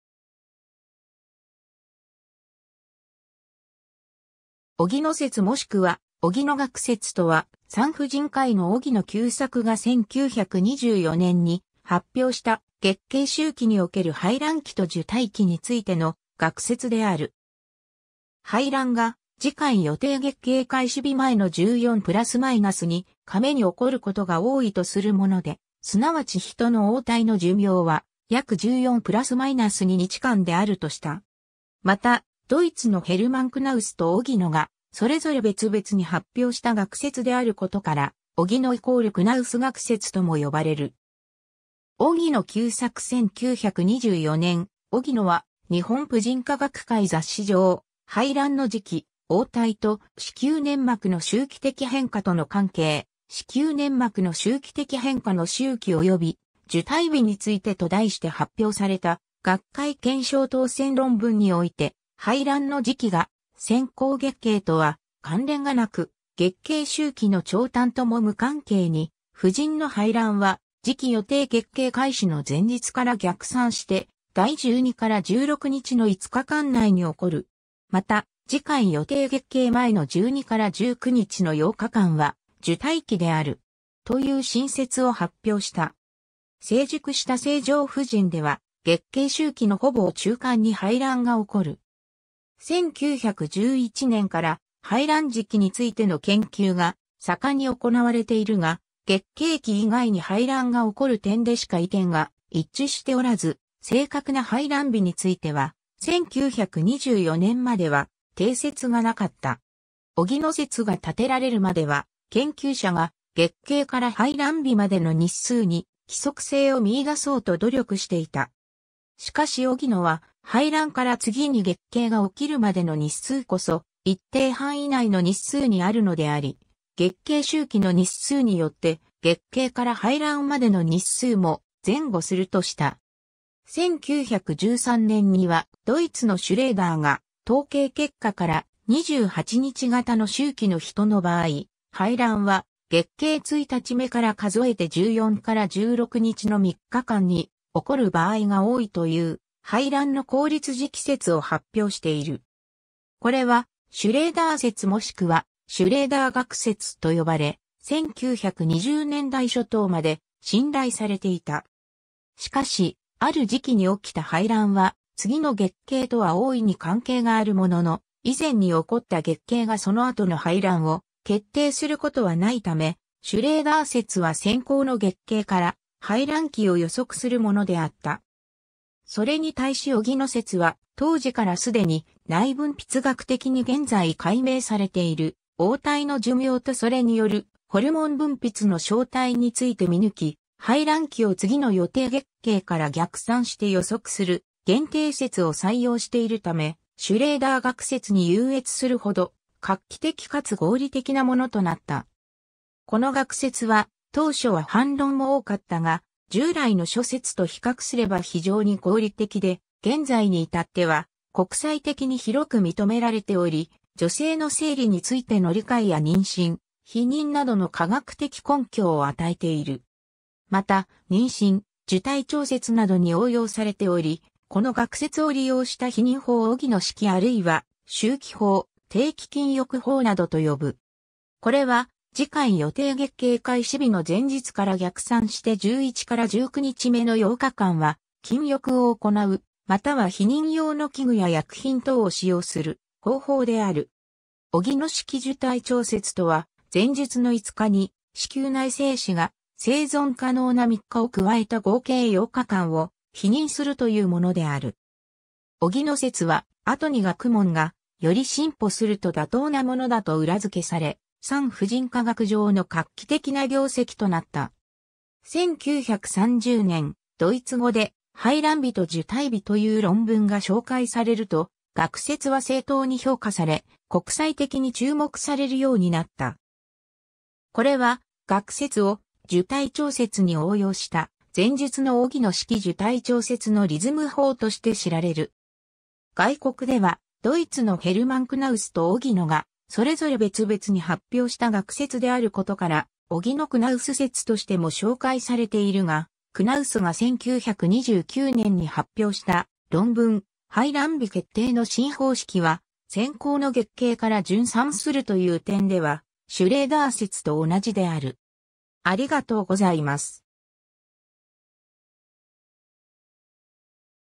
荻野学説
荻野説（おぎのせつ）もしくは 荻野学説（おぎのがくせつ）とは、産婦人科医の荻野久作が1924年に発表した、月経周期における排卵期と受胎期についての学説である。
排卵が、次回予定月経開始日前の14±2日目に起こることが多いとするもので、すなわちヒトの黄体の寿命は約14±2日間であるとした。
また、ドイツのヘルマン・ クナウスと荻野がそれぞれ別々に発表した学説であることから、荻野＝クナウス学説とも呼ばれる。
 荻野久作 1924年、荻野は『日本婦人科学会雑誌 (19巻6号)』上「排卵の時期、黄体と子宮粘膜の週期的変化との関係、子宮粘膜の週期的変化の週期及び受胎日に就て」（原文ママ）と題して発表された学会懸賞当選論文において、「排卵の時期が先行月経とは関連がなく、月経周期の長短とも無関係に、婦人の排卵は、次期予定月経開始の前日から逆算して第12～16日の5日間（14±2日前）内に起こる。また、次回予定月経前の12～19日の8日間は受胎期である。」という新説を発表した。
 成熟した正常婦人では、月経周期のほぼ中間に排卵が起こる。
1911年から、排卵時期についての研究が盛んに行われているが、月経期以外に排卵が起こる点でしか意見が一致しておらず、正確な排卵日については、1924年までは定説がなかった。
荻野説が立てられるまでは、研究者が、月経から排卵日までの日数に規則性を見いだそうと努力していた。
しかし荻野は、排卵から次に月経が起きるまでの日数こそ、一定範囲内の日数にあるのであり、月経周期の日数によって、月経から排卵までの日数も前後するとした。
1913年には、ドイツのシュレーダー（R. Schrder）が、統計結果から「28日型の周期の人の場合、排卵は月経1日目から数えて14～16日の3日間に起こる場合が多い」という、排卵の高率時期説を発表している。
これは「シュレーダー説」もしくは「シュレーダー学説」と呼ばれ、1920年代初頭まで信頼されていた。
しかし、ある時期に起きた排卵は、次の月経とは大いに関係があるものの、以前に起こった月経がその後の排卵を決定することはないため、シュレーダー説は先行の月経から排卵期を予測するものであった。
それに対し荻野説は、当時からすでに、内分泌学的に現在解明されている、黄体の寿命とそれによるホルモン分泌の消退について見抜き、排卵期を次の予定月経から逆算して予測する限定説を採用しているため、シュレーダー学説に優越するほど画期的かつ合理的なものとなった。
 この学説は、当初は反論も多かったが、従来の諸説と比較すれば非常に合理的で、現在に至っては国際的に広く認められており、女性の生理についての理解や妊娠、避妊などの科学的根拠を与えている。
また、妊娠・受胎調節（避妊）などに応用されており、この学説を利用した避妊法をオギノ式あるいは周期法、定期禁欲法などと呼ぶ。
これは、次回予定月経開始日の前日から逆算して11～19日目の8日間は、禁欲を行う、または避妊用の器具や薬品等を使用する方法である。
オギノ式受胎調節とは、前述の5日に、子宮内精子が生存可能な3日を加えた合計8日間を避妊するというものである。
荻野説は、後に学問がより進歩すると妥当なものだと裏付けされ、産婦人科学上の画期的な業績となった。
1930年、ドイツ語で「排卵日と受胎日」という論文が紹介されると、学説は正当に評価され国際的に注目されるようになった。
これは、学説を受胎調節に応用した、前述のオギノ式受胎調節のリズム法として知られる。
外国では、ドイツのヘルマン・ クナウスと荻野がそれぞれ別々に発表した学説であることから、オギノ‐クナウス説としても紹介されているが、クナウスが1929年に発表した論文「排卵日決定の新方式」は、先行の月経から順算するという点では、シュレーダー説と同じである。